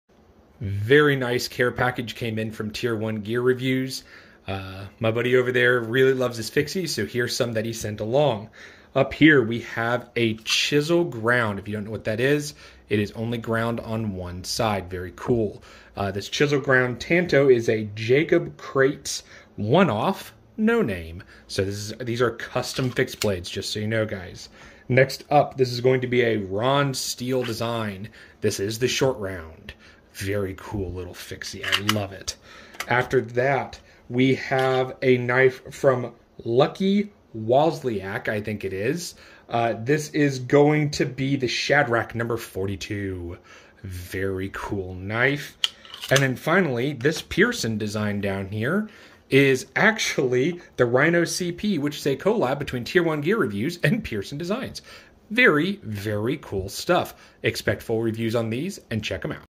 SO MANY CUSTOMS
So many custom fixed blades from by buddy @Tier1GearEDCReviews

Logo from @grayscale_carry on Instagram